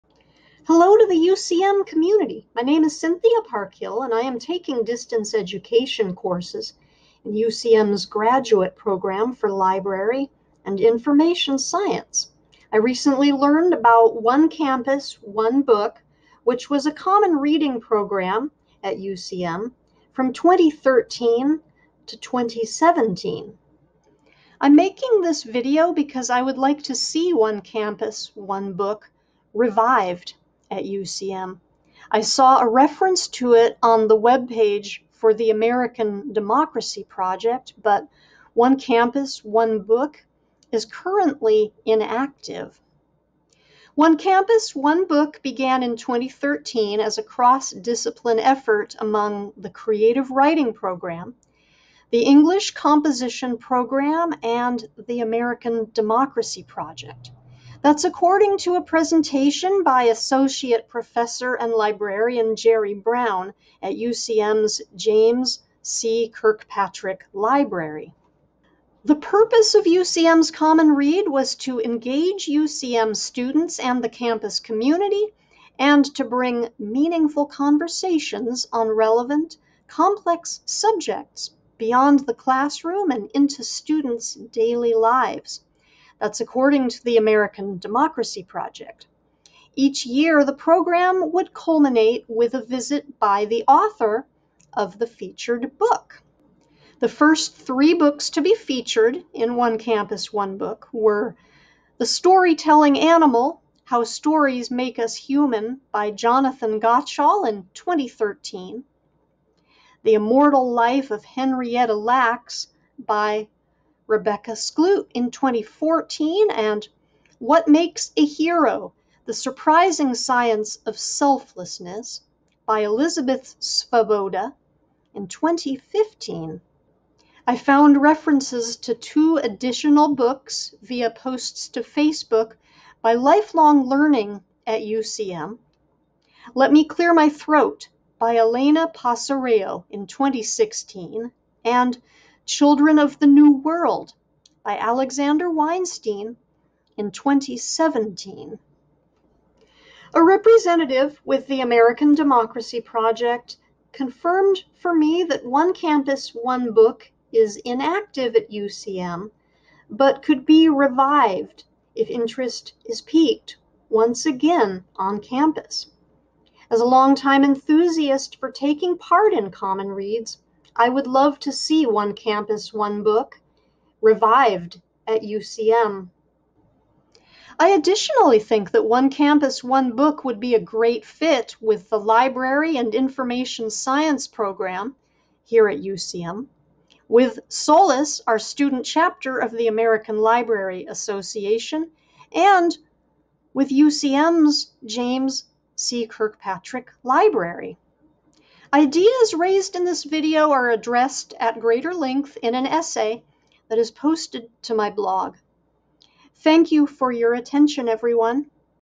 ‘One Campus, One Book’ at UCM
“One Campus, One Book” was a common reading program at the University of Central Missouri from 2013 to 2017. I made this video because I would like to see “One Campus, One Book” revived at UCM. I think that One Campus, One Book would be a great fit with the Library and Information Science program here at UCM; with SOLIS, our student chapter of the American Library Association; and with UCM’s James C. Kirkpatrick Library.